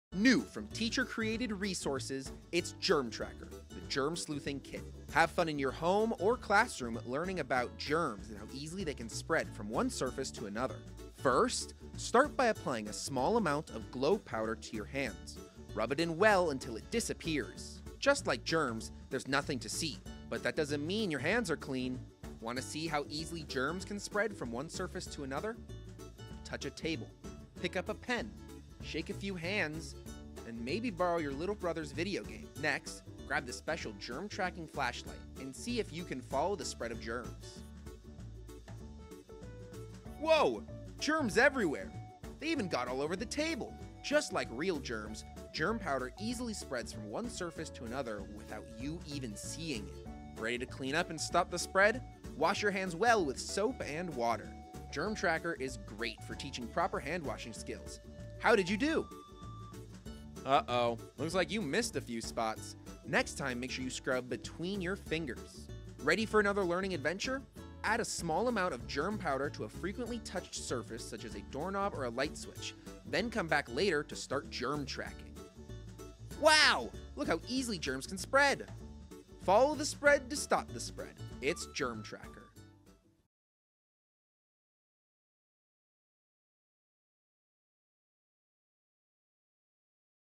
Germ Tracker: The Germ-Sleuthing Kit
Follow the spread! Have fun while learning about how germs spread and the importance of proper hand washing. This easy-to-use kit provides safe interactive tools for demonstrating how easily harmful bacteria and viruses can transfer from one surface to another.